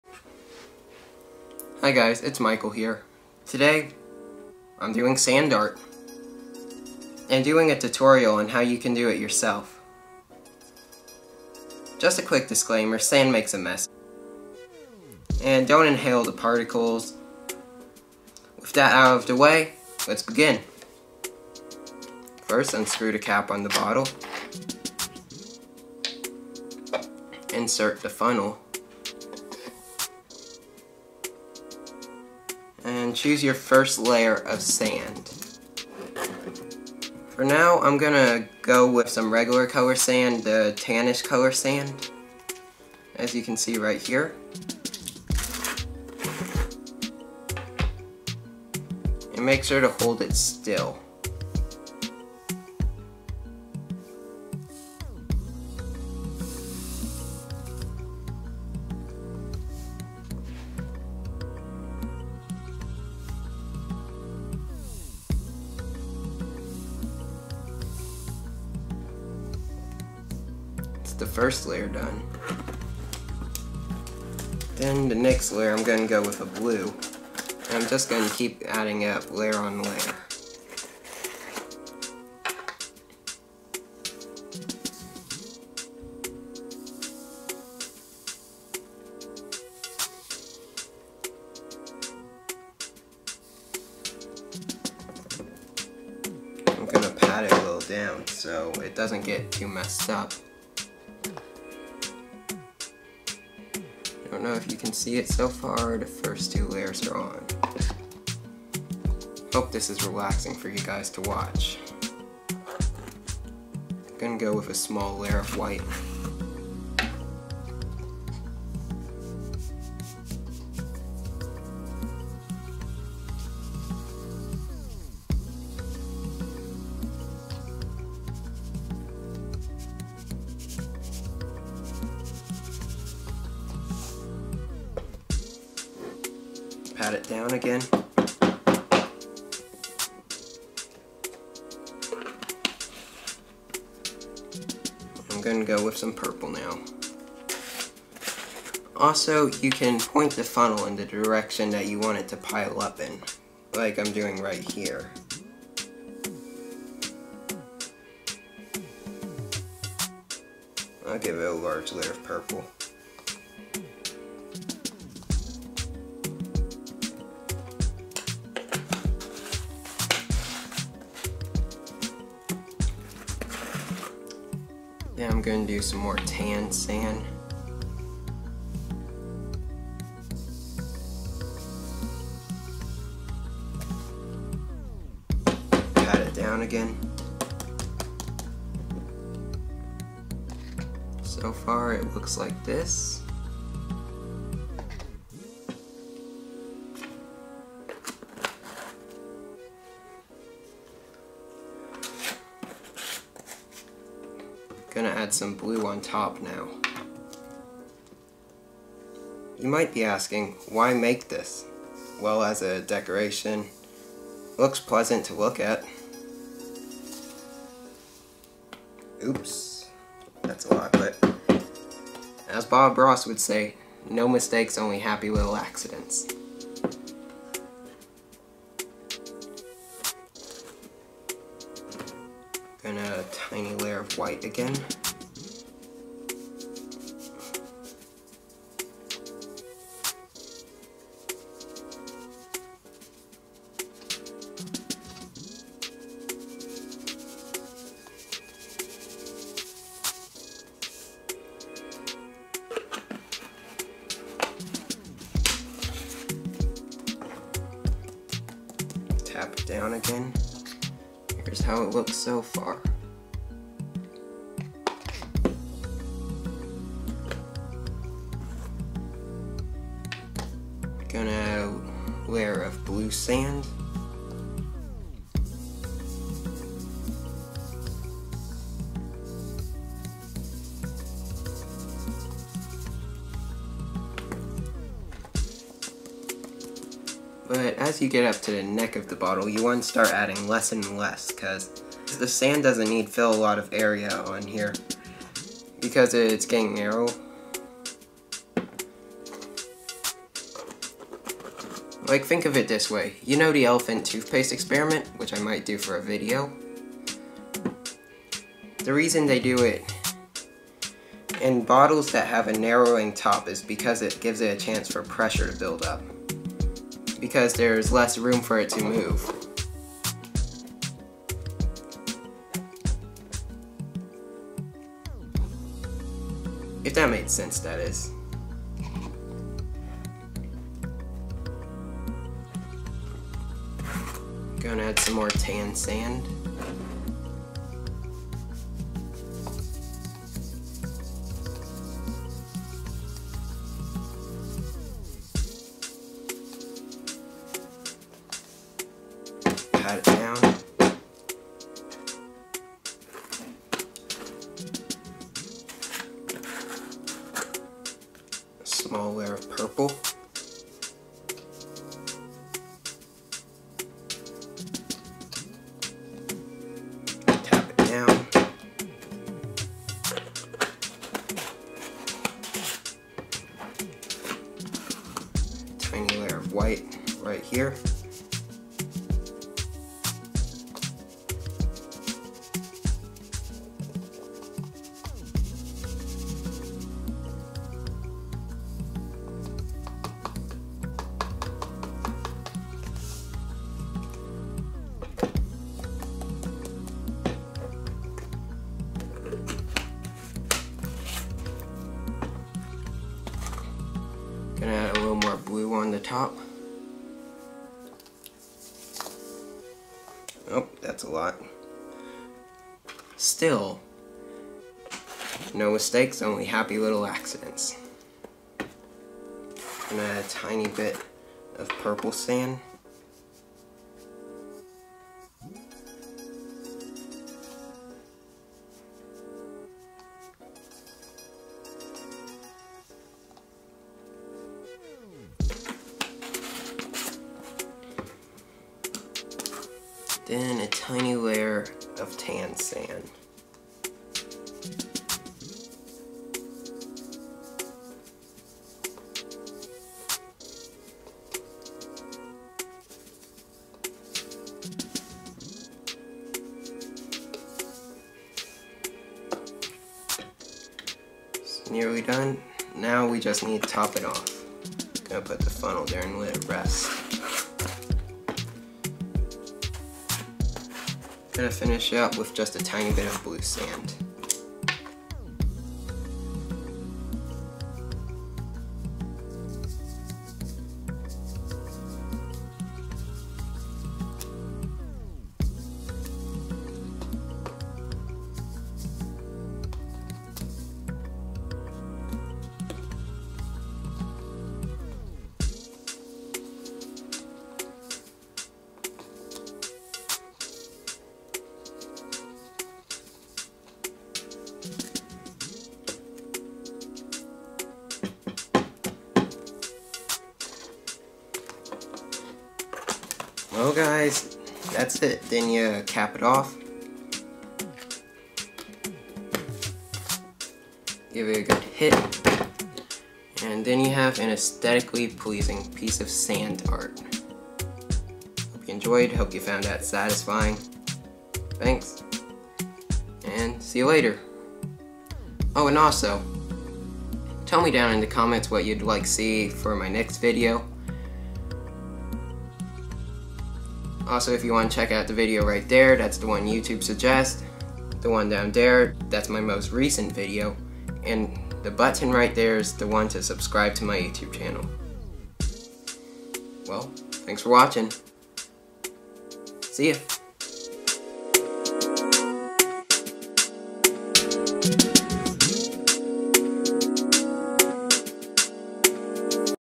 Satisfying sand art, and how to make it.
In this video I will show you how to make extremely beautiful sand art that is pleasant to look at and also is a great decoration.